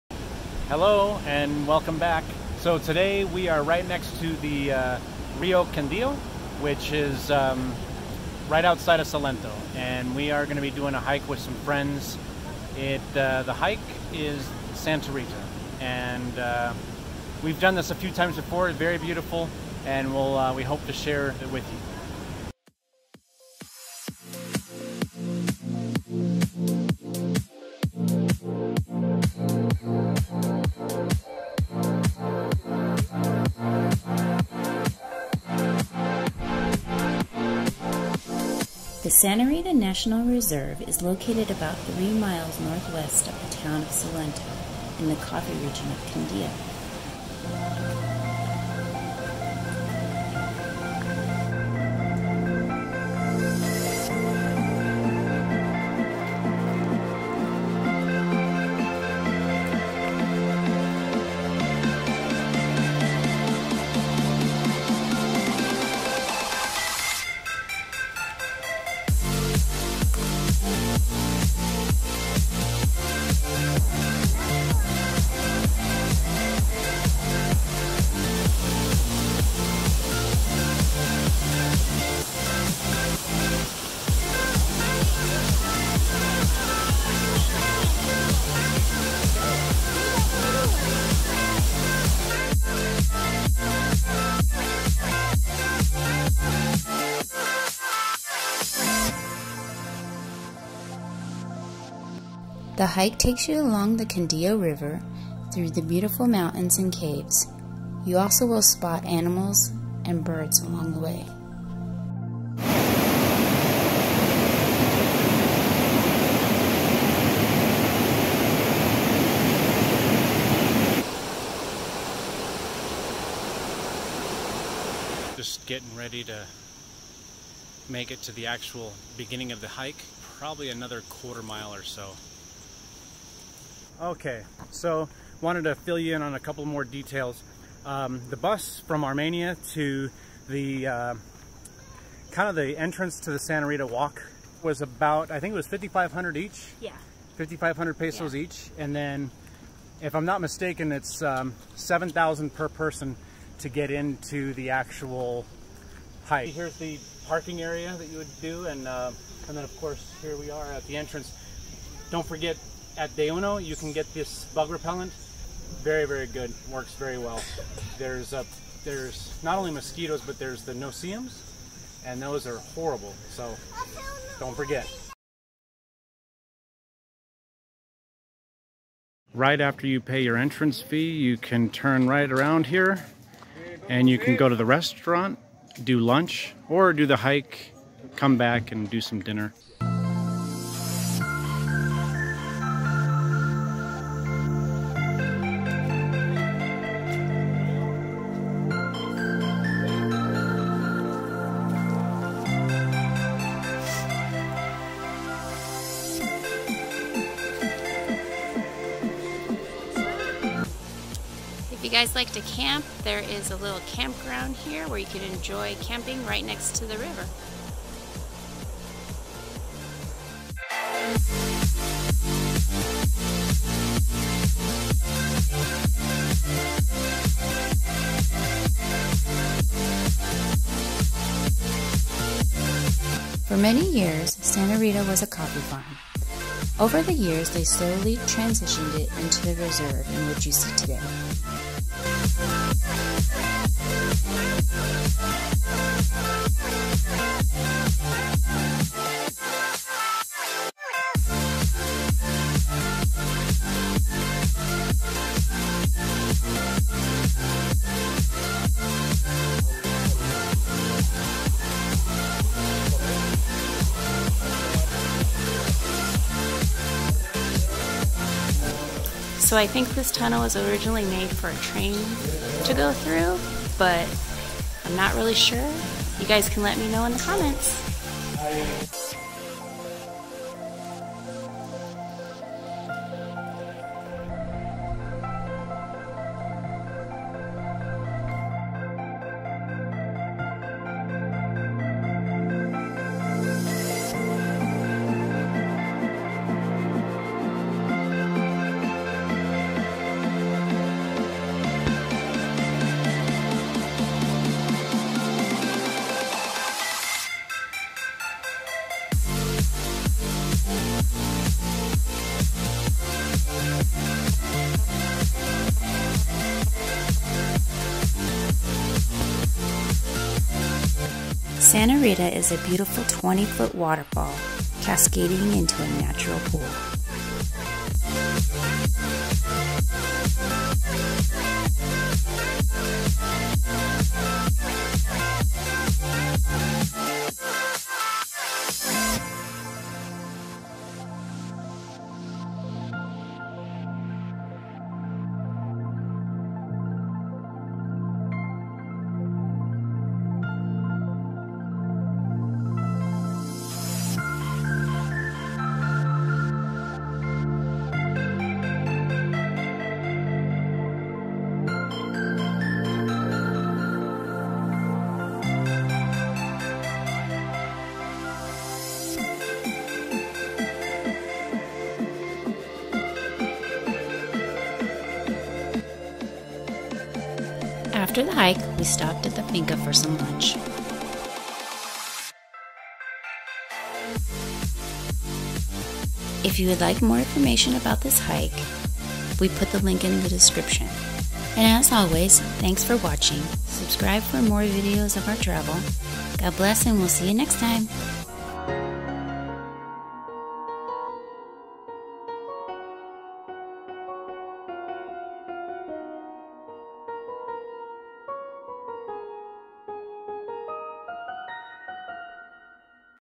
This Hike is AMAZING!!!! Santa Rita La Cascada - Salento, Quindío, Colombia
This hike can be as short or as long as you want. Our friends had young children, so we didn't stay out long. There are several other hikes you can take once you see the first waterfall. Also, there is a great lookout on one of the hikes that allow you to see clear out to Armenia, if it's a clear day. This hike is one of thousands you can enjoy while in Colombia.

Rates
Daily Pass - $ 7.000 cop
Camping - $ 20.000 cop
                   per person / per night
Finca Stay - $ 55.000 cop

Consider Supporting our channel!